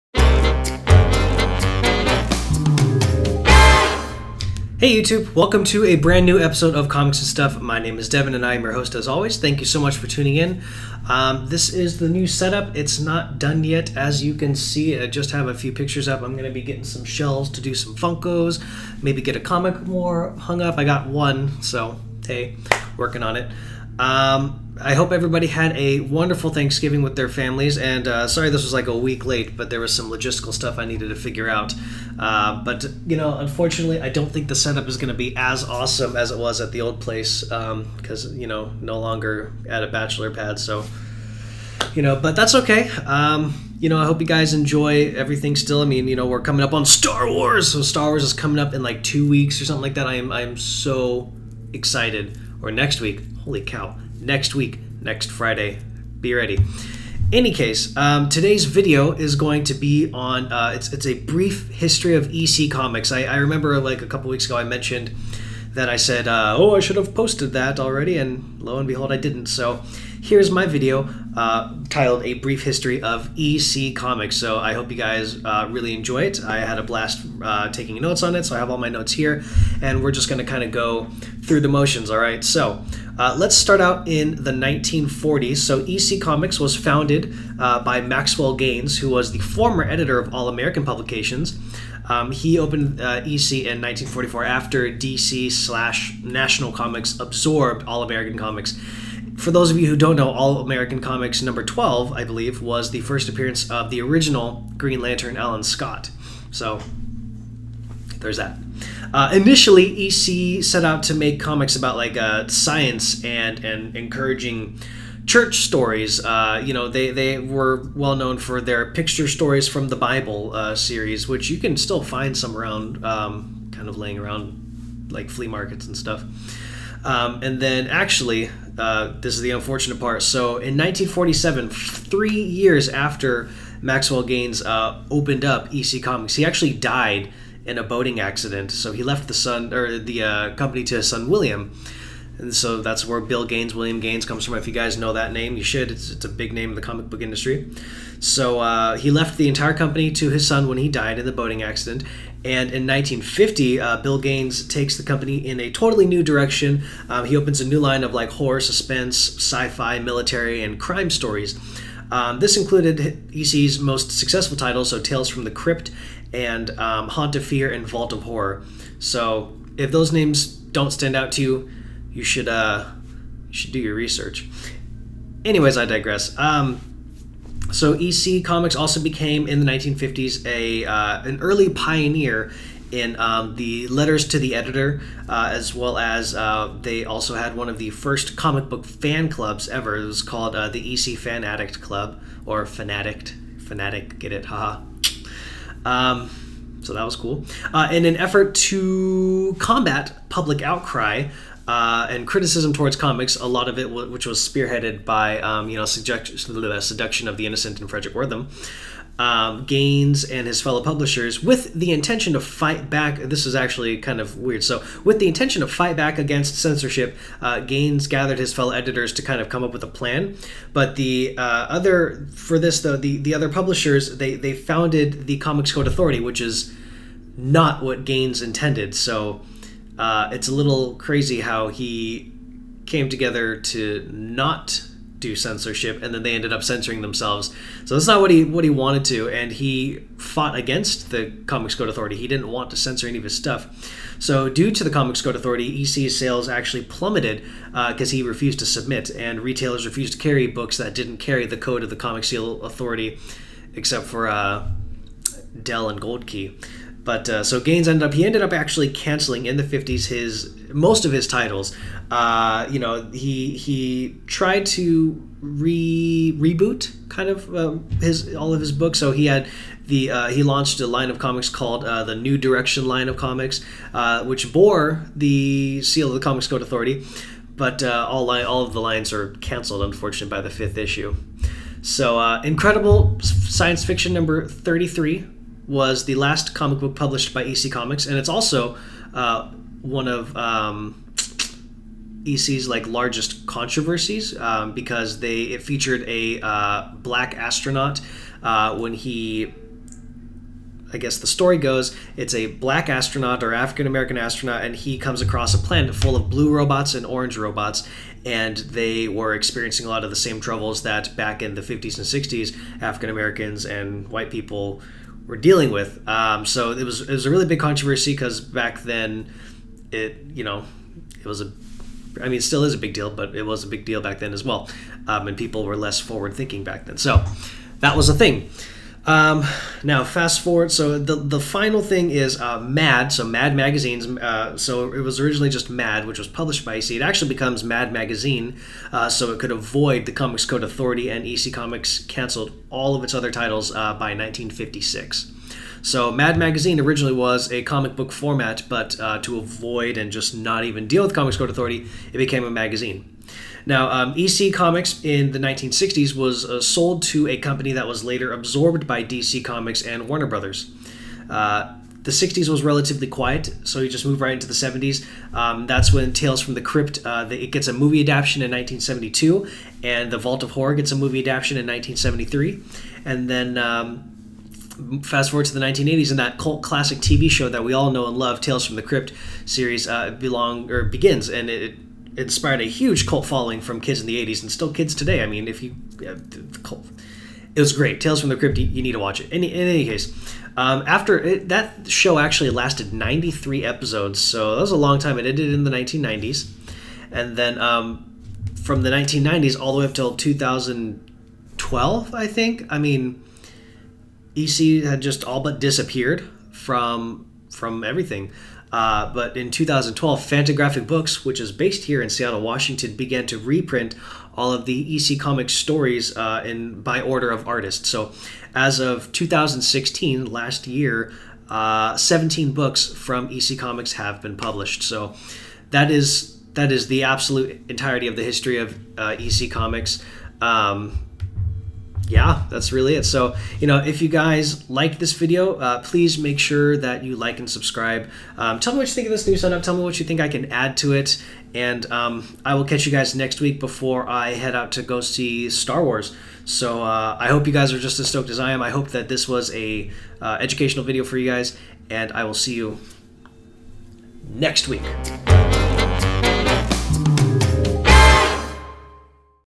A Brief History of EC Comics | Comic Histories
So sorry for such a late upload! Ever curious about EC Comics? Here is my briefly summed up history. I hope you enjoy! Make sure to give the Instagram a ton of love @Comics_And_Stuff1 and don't forget to check out the Comics 'n Stuff RedBubble page to support!